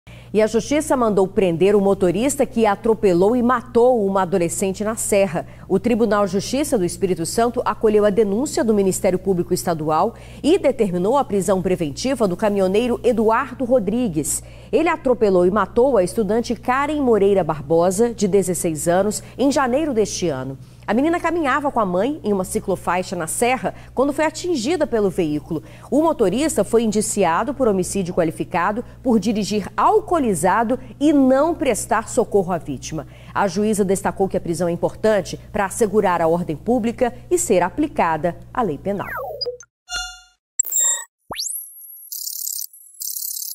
Justiça determina prisão de motorista que atropelou e matou adolescente no ES
Karen Moreira Barbosa, de 16 anos, foi atropelada enquanto caminhava com a mãe em uma ciclofaixa no bairro Jacaraípe, na Serra.